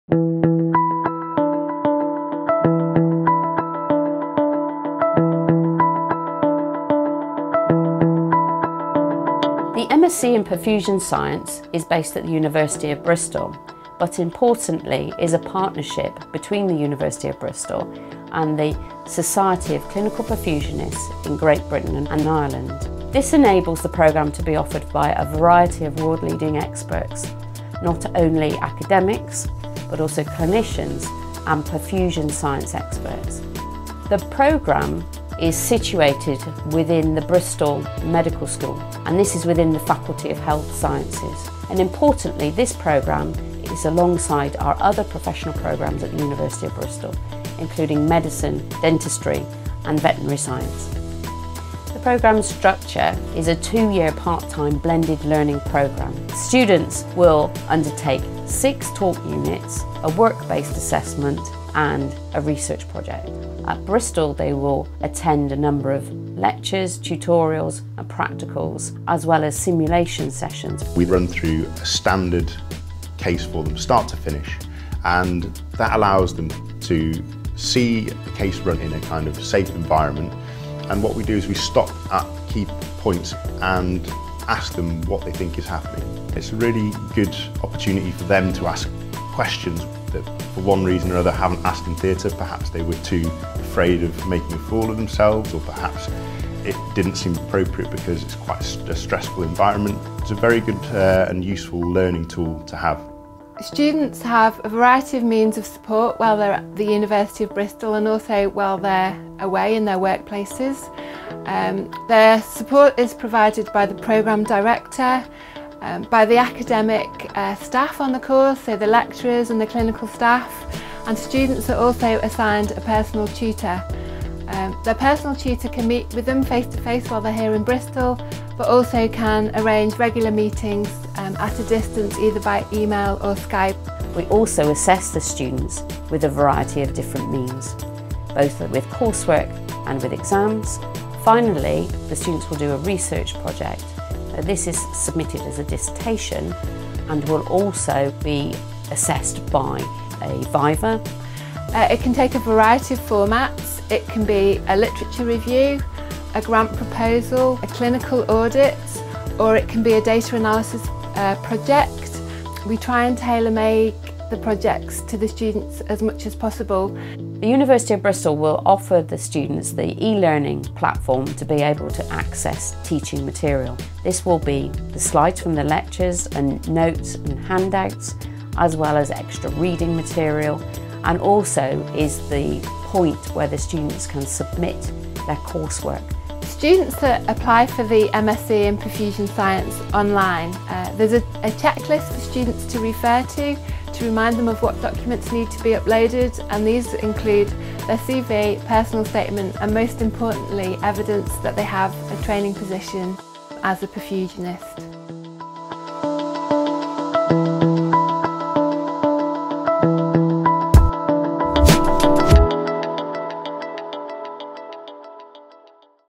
Why choose an MSc in Perfusion Science at Bristol?
An introduction to the MSc Perfusion Science at the University of Bristol